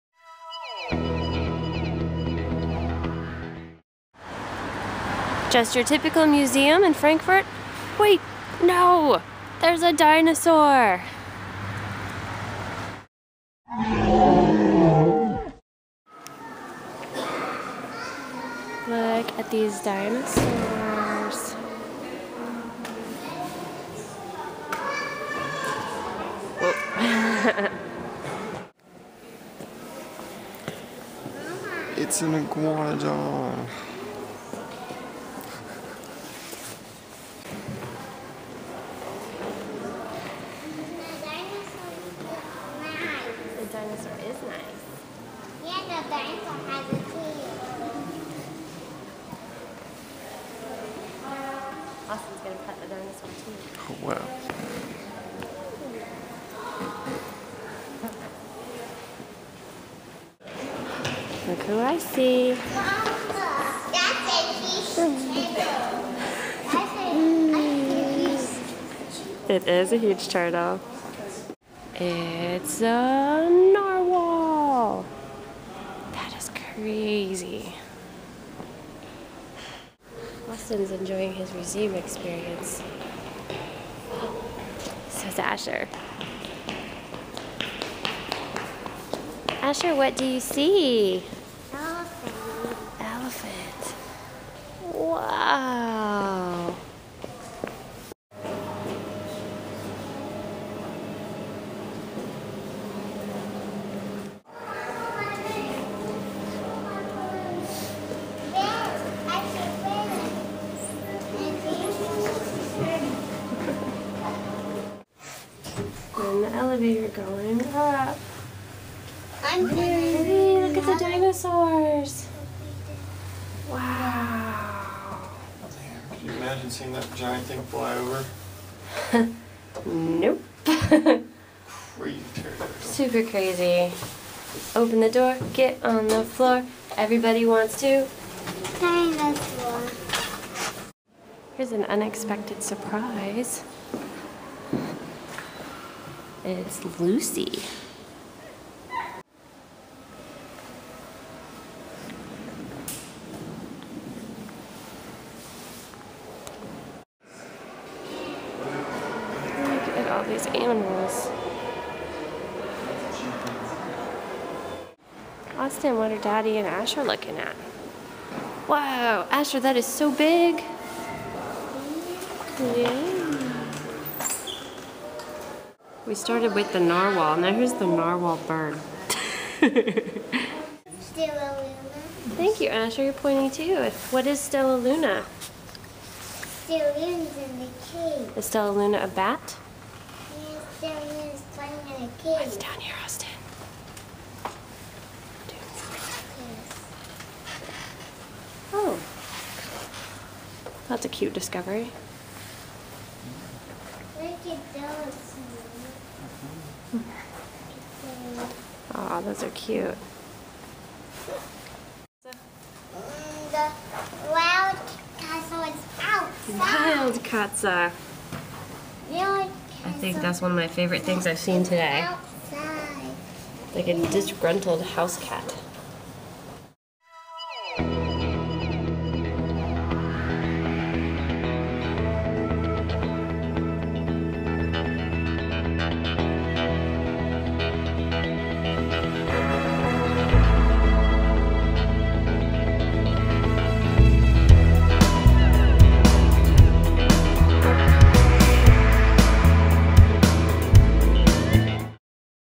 Naturmuseum Senckenberg, Frankfurt
We thoroughly enjoyed this natural history museum complete with dinosaurs, mummies, animals, Lucy and more!  Here is some footage from our wonderful visit.  We will definitely return!